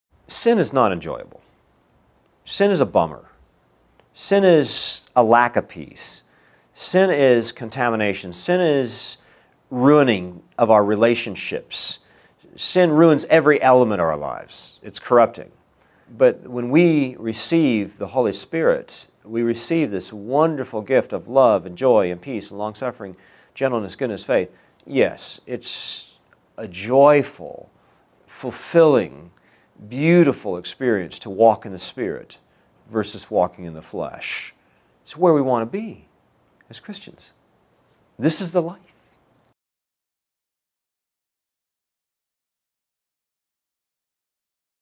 What's So Bad About Sin?
Does anything good result from a life that is given over to sin?

Kevin Swanson explains in this video that sin is not enjoyable. Sin results in a lack of peace. Sin ruins our relationships. In fact, sin ruins and corrupts every element of our lives. In short, there is no good that comes from a life of sin.

In contrast, when we receive the Holy Spirit, we receive the fruits of the Spirit. It is a joyful, fulfilling experience to walk in the Spirit as opposed to walking in the flesh. It is where we want to be as Christians. And yet, sadly, countless people wrongly believe that the better life is lived by indulging in sin and all the world has to offer. For Christians, the best is yet to come – eternal life.

Romans 6:23 (NKJV) – “For the wages of sin is death, but the gift of God is eternal life in Christ Jesus our Lord.”